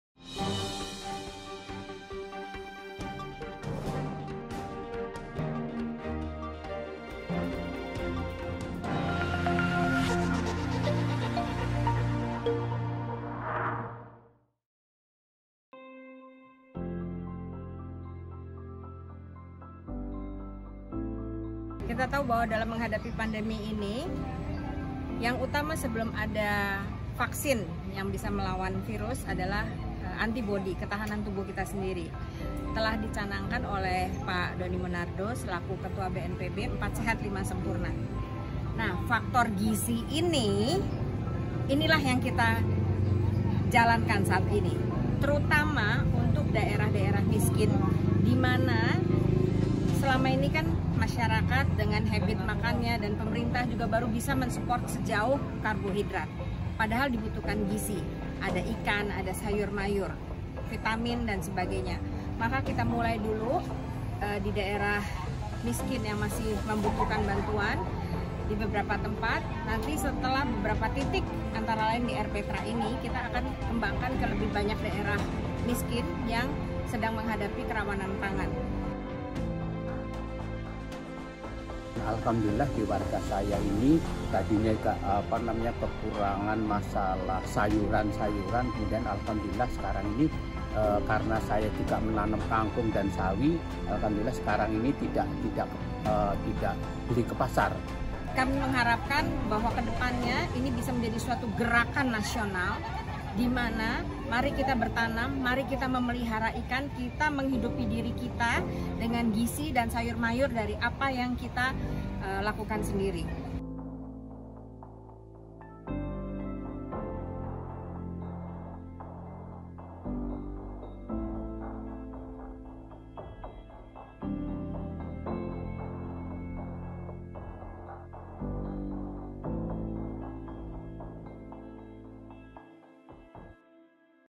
Urban Farming di Masa Pandemi
Halo SahabatTangguh.
'Urban farming’ adalah satu metode pertanian kota dengan konsep berkebun di lahan terbatas. Hasilnya diharapkan dapat memenuhi kebutuhan pangan rumah tangga, juga menunjang perekonomian masyarakat itu sendiri melalui pemasaran hasil panen urban farming.

Indonesia Against Covid -19 (IAC-19) merupakan satu komunitas dari para penggiat ‘urban farming’ di Indonesia. Salah satu kegiatan yang dilakukan dan terbukti berhasil adalah Budikdamber atau budidaya kangkung dan pembesaran ikan lele dalam ember.

Ini bisa menjadi alternatif bagi warga kota dalam mengisi waktu luang di rumah selama pandemi. Kegiatan budikdamber juga cocok diterapkan lintas masyarakat, khususnya bagi mereka yang kesulitan dalam memenuhi kebutuhan pangan selama pandemi covid-19. Kegiatan itu juga memberi kesibukan dan pengalaman baru bagi pelakunya.

Konsepnya sangat sederhana dan tak butuh banyak modal. Cukup siapkan satu ember berkapasitas 60 liter agar bisa memuat lebih dari 60 ekor lele saat panen. Selain ember, barang lain yang dibutuhkan adalah beberapa pot plastik air mineral, media tanam seperti arang, sekam dan lainnya serta kawat jemuran untuk pengait pot yang mudah didapat dari limbah rumah tangga.

Bila program ‘urban farming’ diikuti oleh masyarakat banyak, maka hasilnya akan berdampak langsung pada masyarakat. Kegiatan ini diharapkan dapat membantu pemerintah dalam menjaga ketahanan pangan. Selain menguatkan rasa kebersamaan dan budaya gotong royong di masyarakat.

Beberapa waktu lalu, Ketua Satgas Penanganan Covid-19 Doni Monardo mengenalkan slogan 4 Sehat 5 Sempurna diera Korona. Doni mendefinisikan 4 Sehat 5 Sempurna sebagai upaya mencegah Covid-19. Empat sehat dari Doni yang juga Kepala BNPB, yaitu gunakan masker, jaga jarak, baik physical dan social distancing, rajin cuci tangan dengan sabun, dan olahraga, tidur teratur dan cukup, serta tidak panik. Satu pesan yang membuat sempurna yaitu makanan yang bernutrisi.